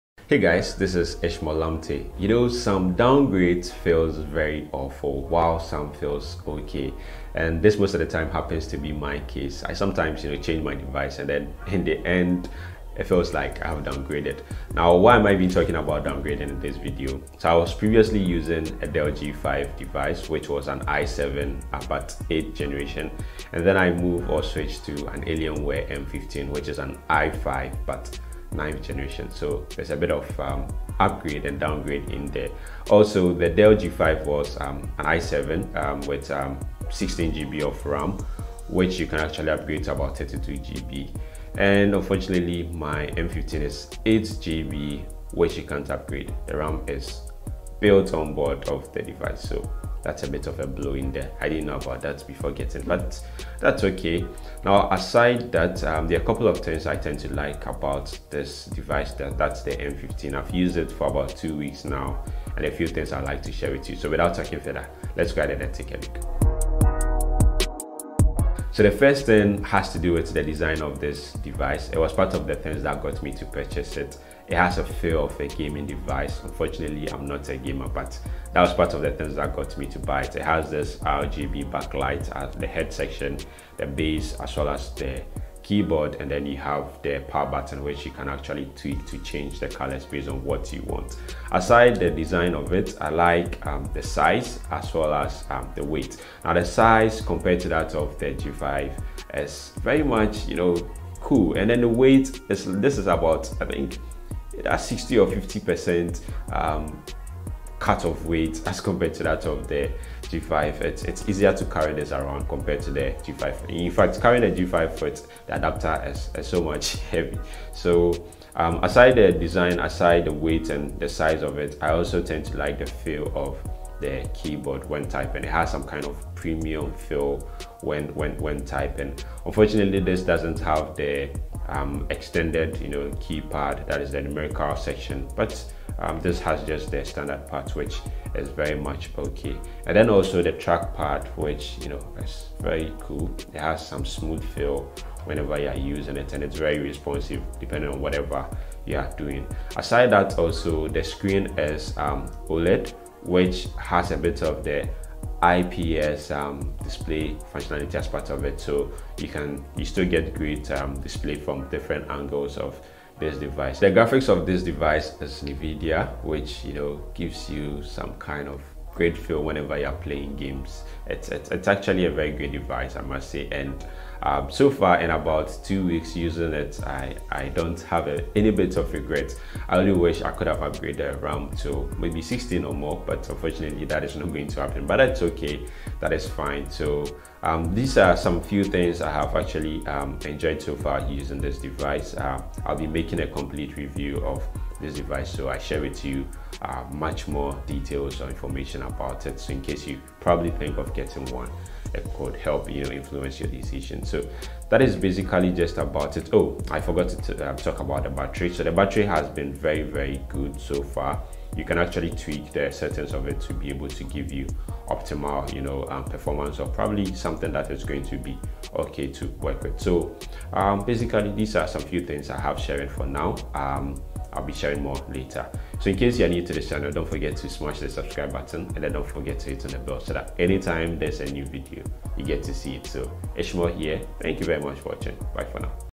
Alienware M15 R2 Gaming Laptop Impression
Here's a short impression on the Alienware M15 R2 Gaming Laptop from dell. In this video, I share my impression so far after using it for about 2 weeks. I talk about the M15 design, the keyboard, the display, the size, and weight as well as a few specs of it.

My Dell G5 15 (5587) core i7 gaming laptop Review